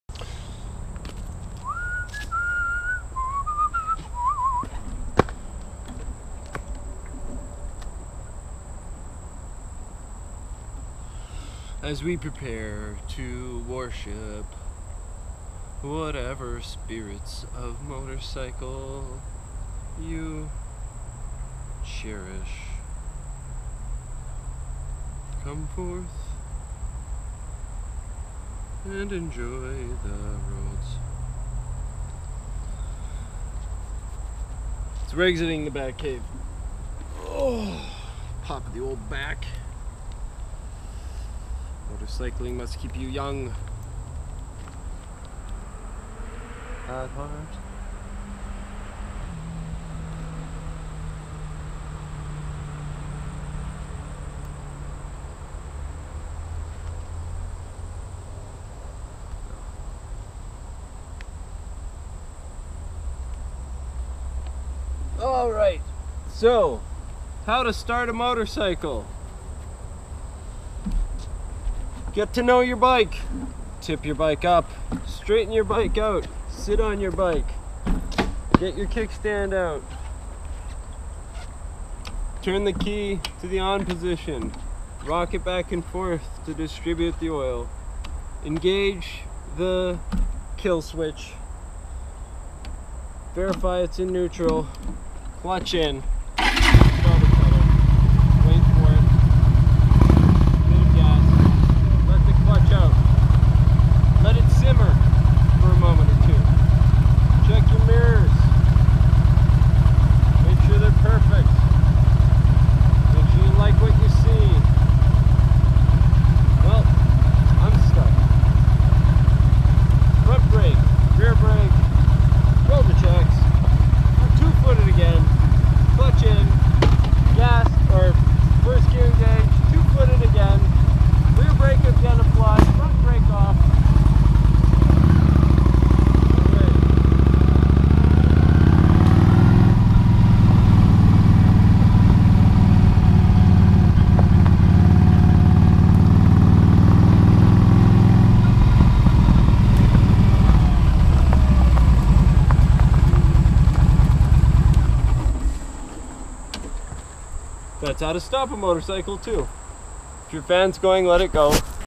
How to Properly Start a Motorcycle
Professional motorcycle start procedure. As detailed as it gets.